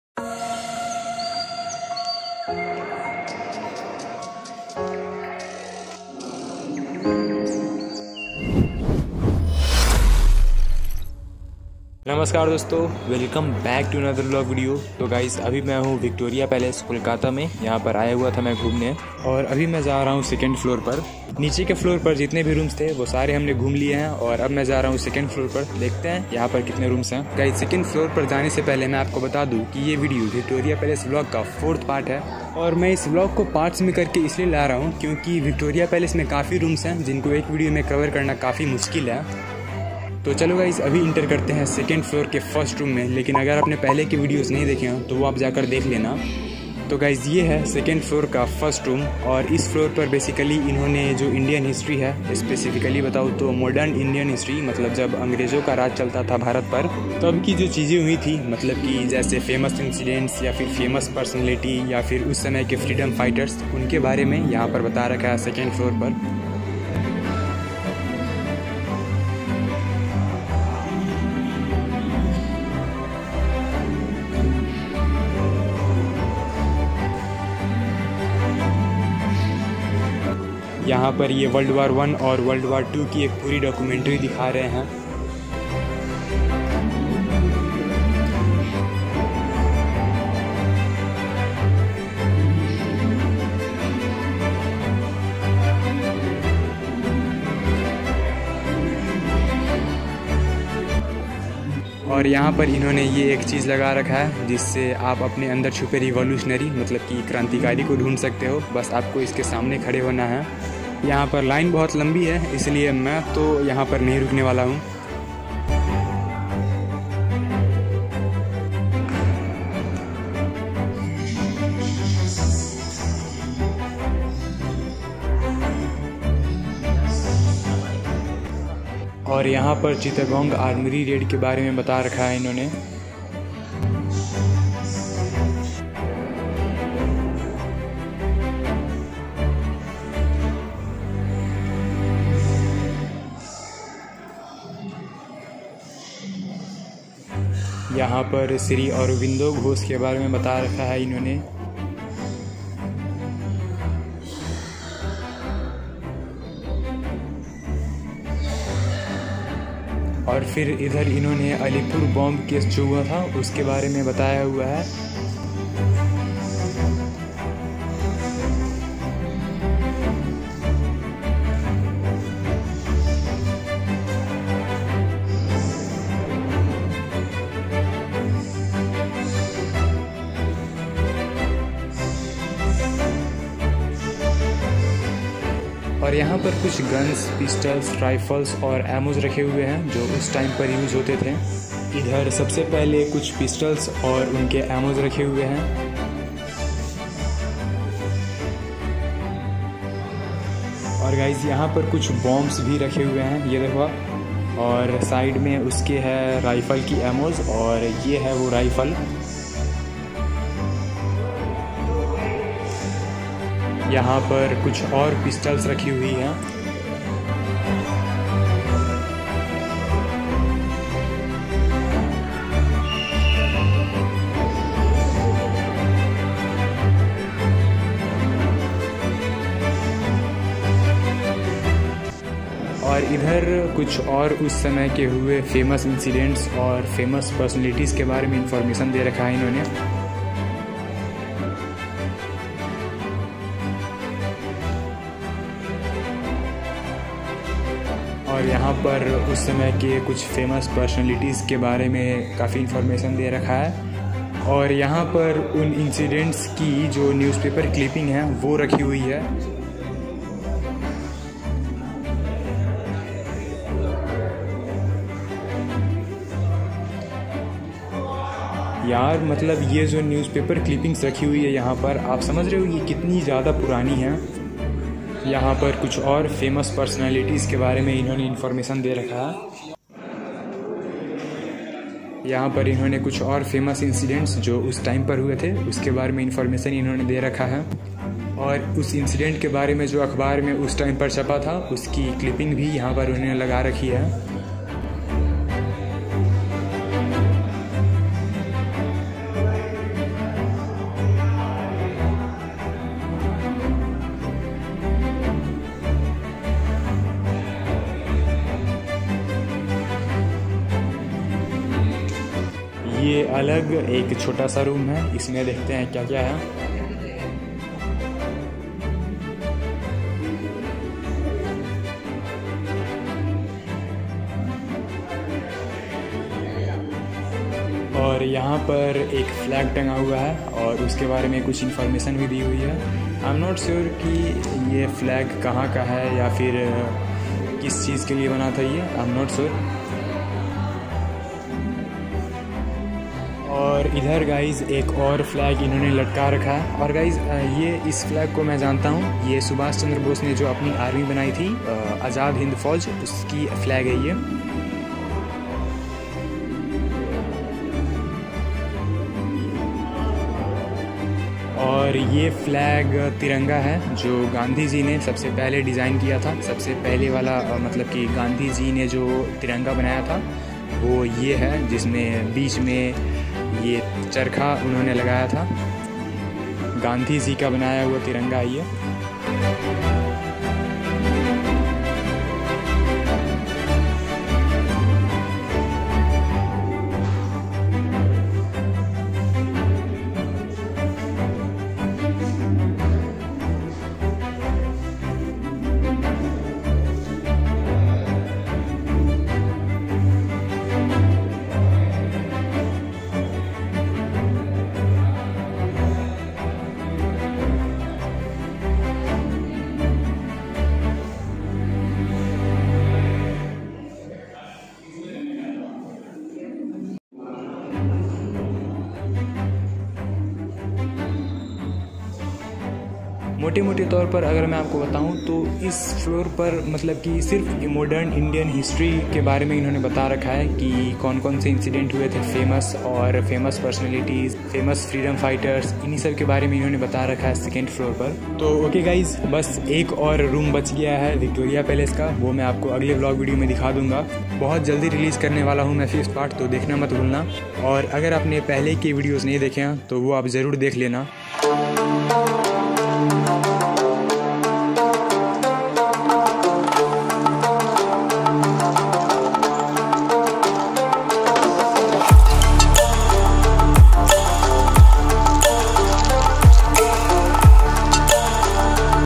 Secrets of THE VICTORIA PALACE | vlog | Aditya prakash vlogs
Join us on an intriguing journey through Victoria Palace in Kolkata, where we unveil the hidden truths of Indian history. Discover the lesser-known stories and secrets embedded within this architectural marvel. From its majestic design to the untold tales of the British Raj, this video reveals fascinating insights about India's colonial past. Don't miss this deep dive into the mysteries of one of Kolkata's most iconic landmarks.